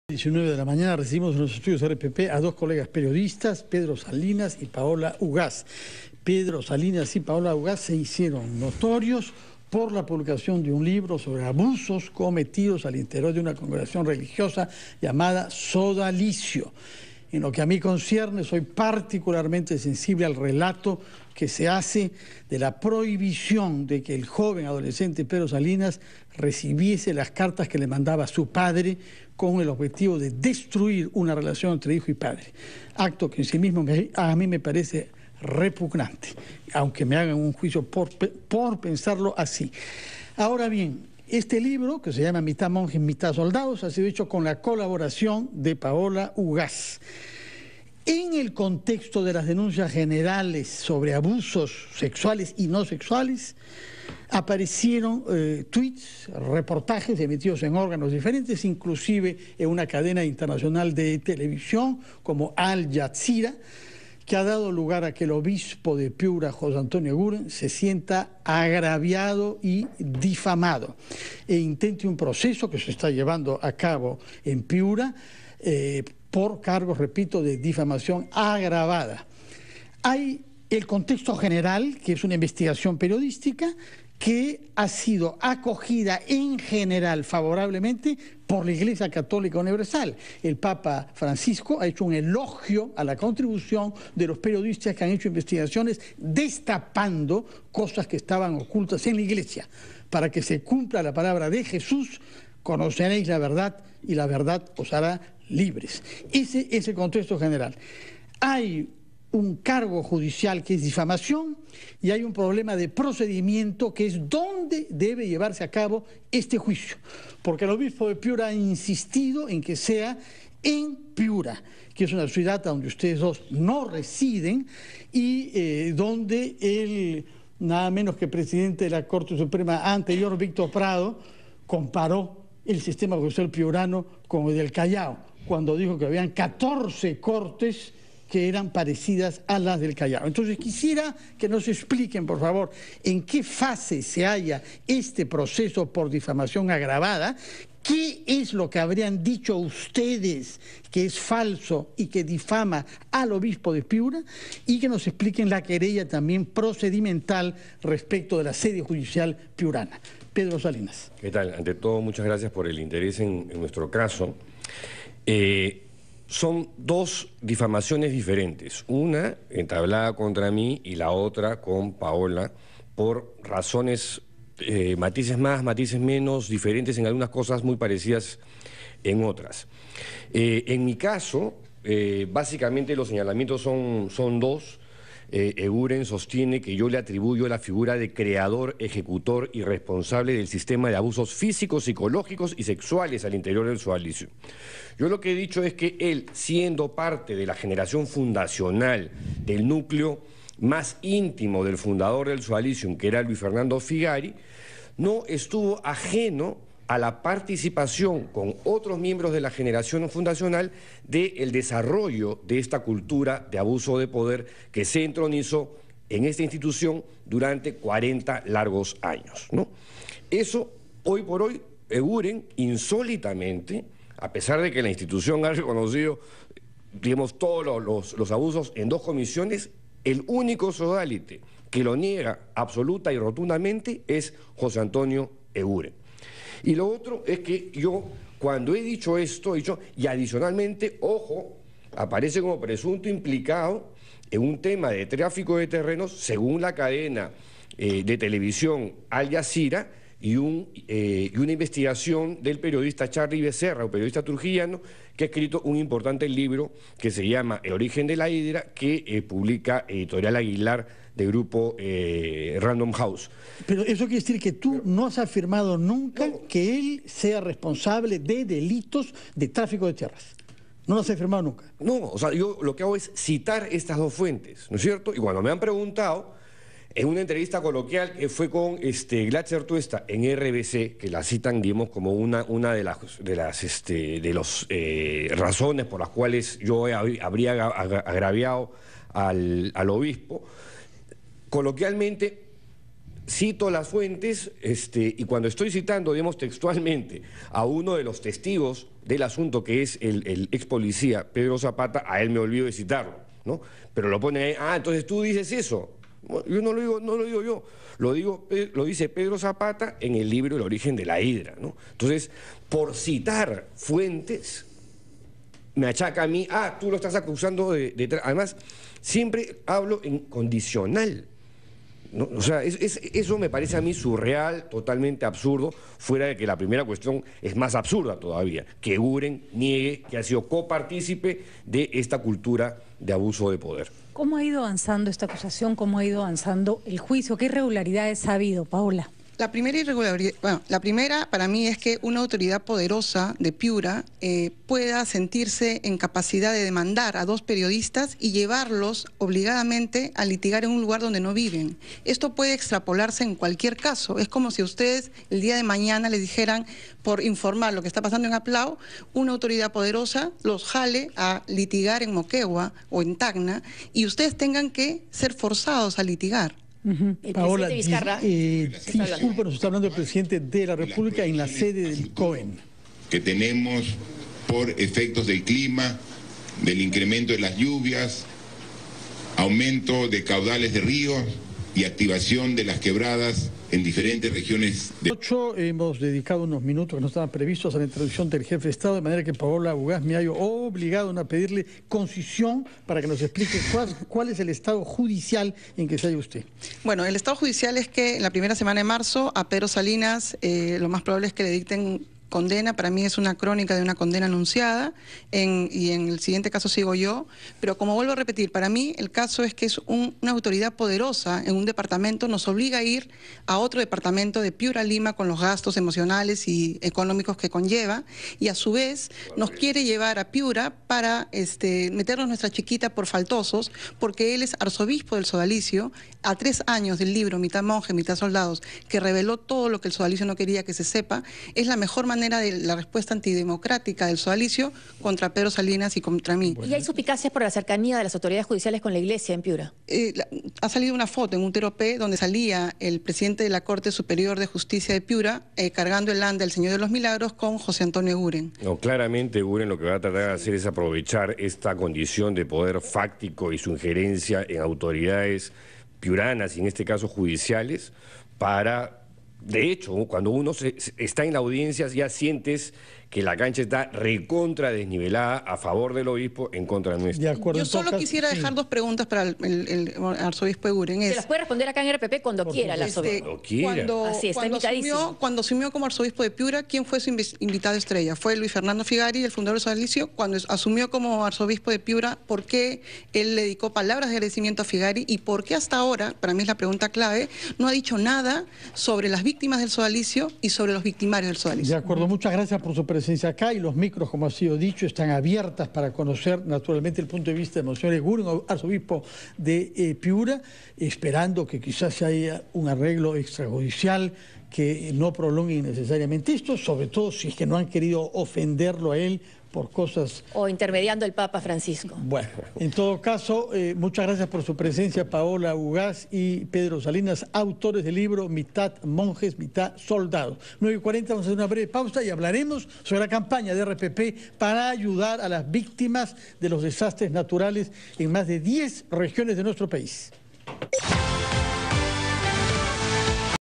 Entrevista a Pedro Salinas y Paola Ugaz sobre denuncias de arzobispo Eguren - RPP - 11-02-19
El periodista Pedro Salinas dijo que tiene dos denuncias por difamación de parte del obispo de Piura, una de ellas en la que se señala que ha creado un sistema de abusos de parte del Sodalicio y que pese a citar una fuente, también se le puso un agravante adicional. En tanto, la periodista Paola Ugaz indicó que está sorprendida por el hecho de que una autoridad poderosa de Piura, los lleve a ese lugar a afrontar el juicio que tiene en su contra y que la publicación del libro “Mitad monjes, mitad soldados” recogió los testigos del caso.